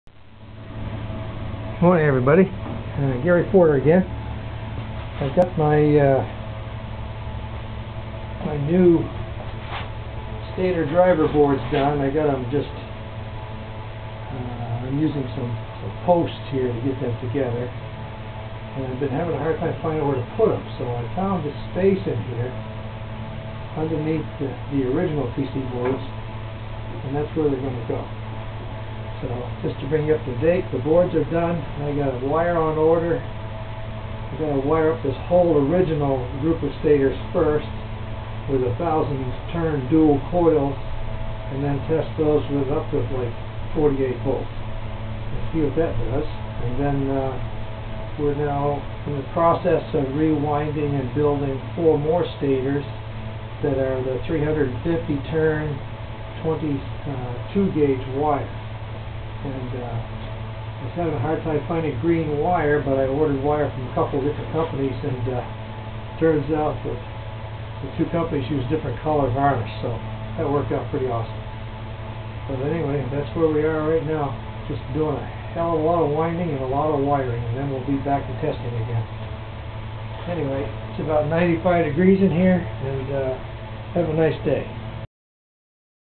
Don Smith 160update on impulse motor
Just finished the new PCB's that will drive each of the stators individually.  Also I am adding a power switch for each stator  pos & neg power supply so that I can engage  them one at a time.  also building the new set of stator s using dual 350 turns .  this is very close to the old prototype setup.   just waiting on wire lots of it.  The video shows how the driver PCB;s will be located.